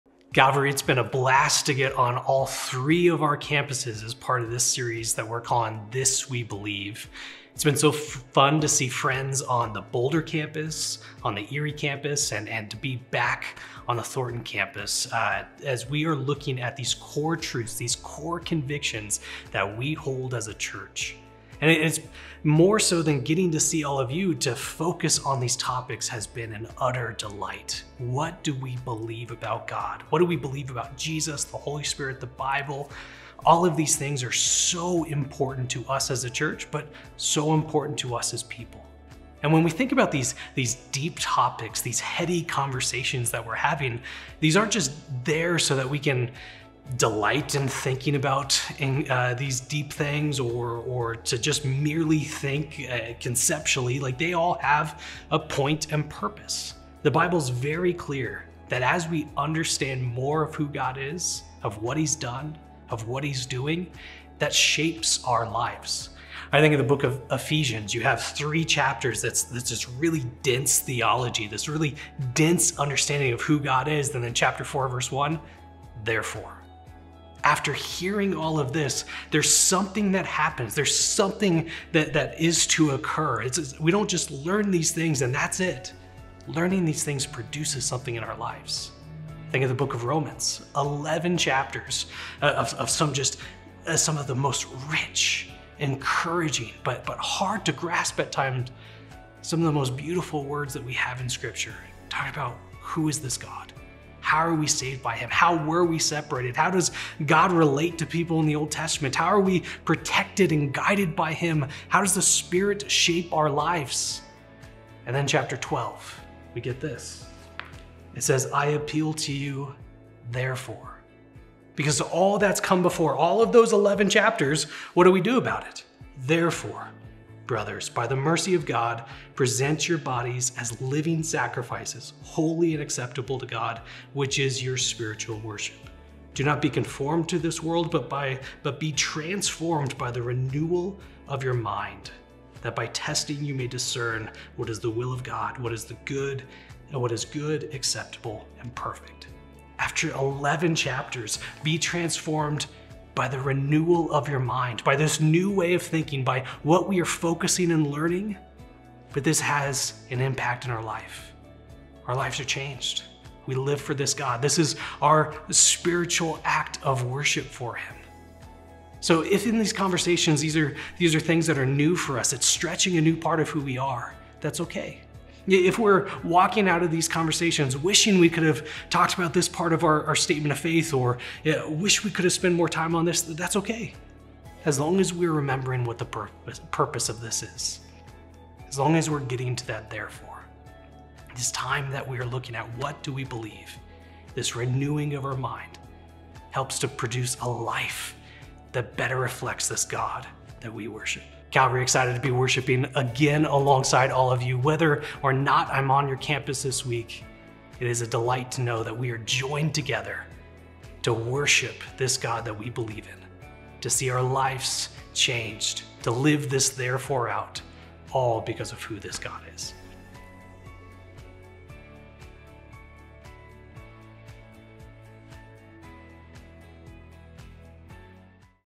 Renewing Our Minds | June 28, 2023 | Calvary Bible Church | Boulder, Erie, Thornton, Colorado
Zack shares how we should allow the content of the This We Believe series to renew our minds.

Keep up with all the events happening at Calvary by checking out